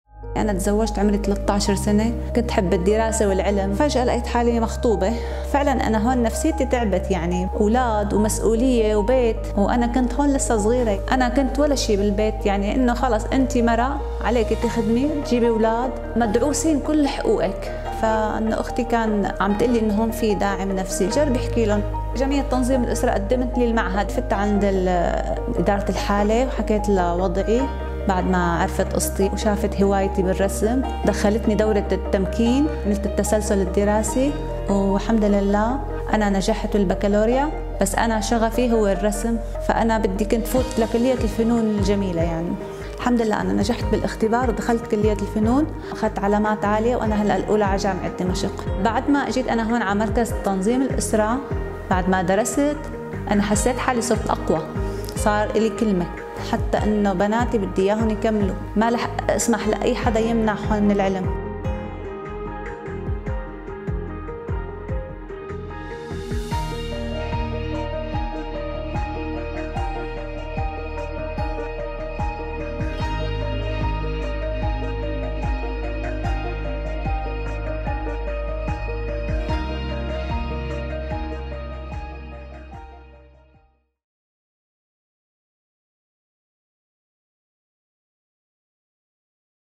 Unlocking the inner strengths
In Syria, at the tender age of 13, Samah was forced into marriage. Years of feeling unworthy plagued her, but with the irreplaceable assistance she found in a UNFPA-supported Women and Girls Safe Space, she embarked on a journey of self-discovery, unlocking her inner strength and reaching her full potential.